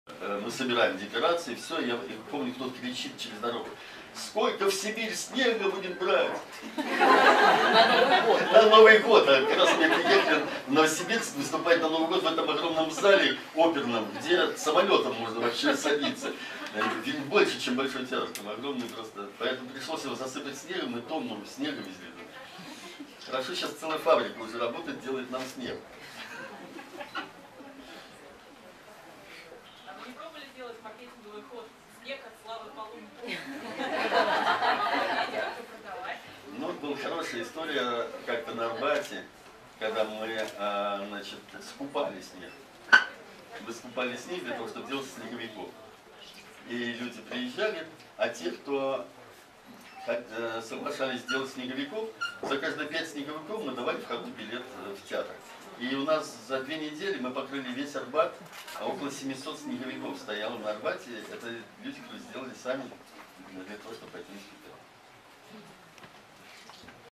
Slava Polunin in Moscow: Stairway to Heaven Book Presentation
The world-known and much-loved clown, Slava Polunin, presented his new book, Stairway To Heaven, at the Biblio-Globus bookstore in Moscow on June 20, 2012. The book is a collection of photographs taken at various Carnival projects heralded by Slava.